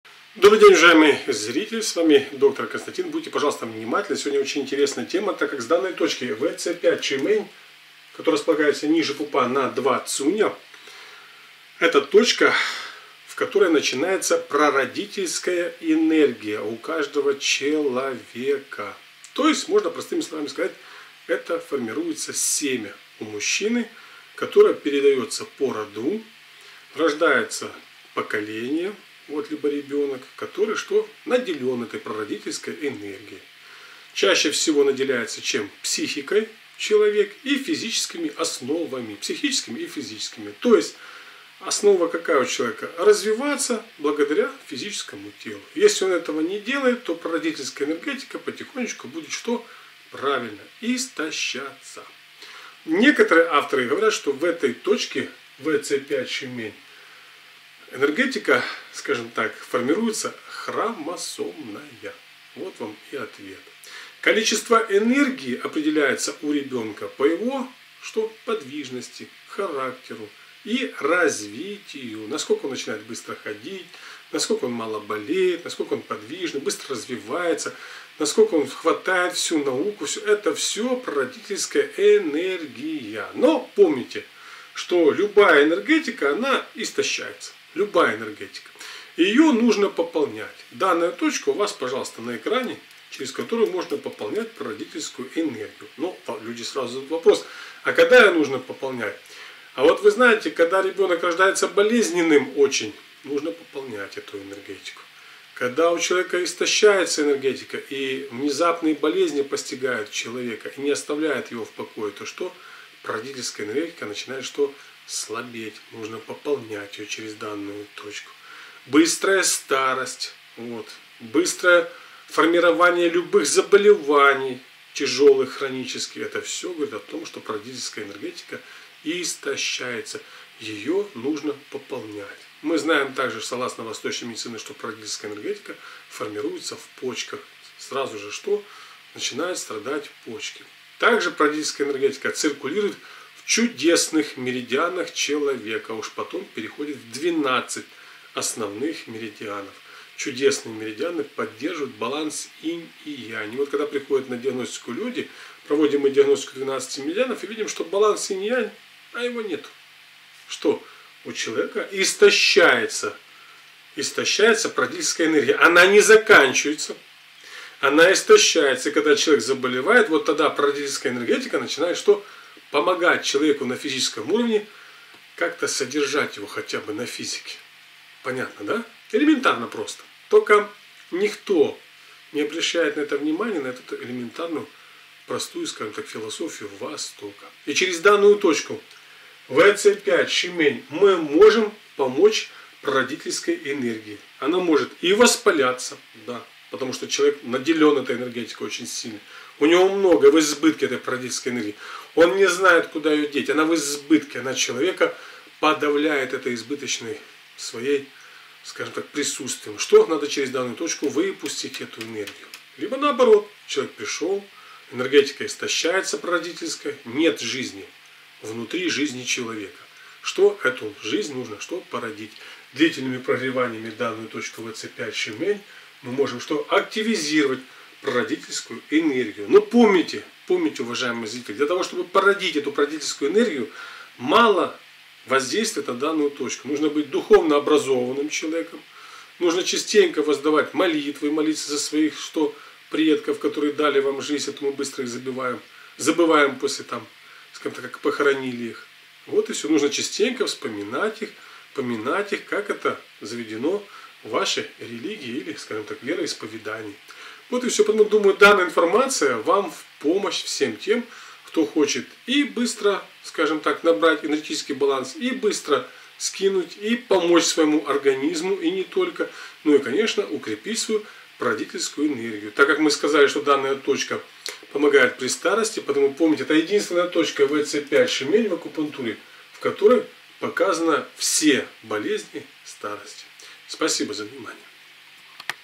Точка Прародительской ЦИ.
Точка Пополнения Прародительской Энергией. Видео-Урок Иглотерапевта Доктора Константина.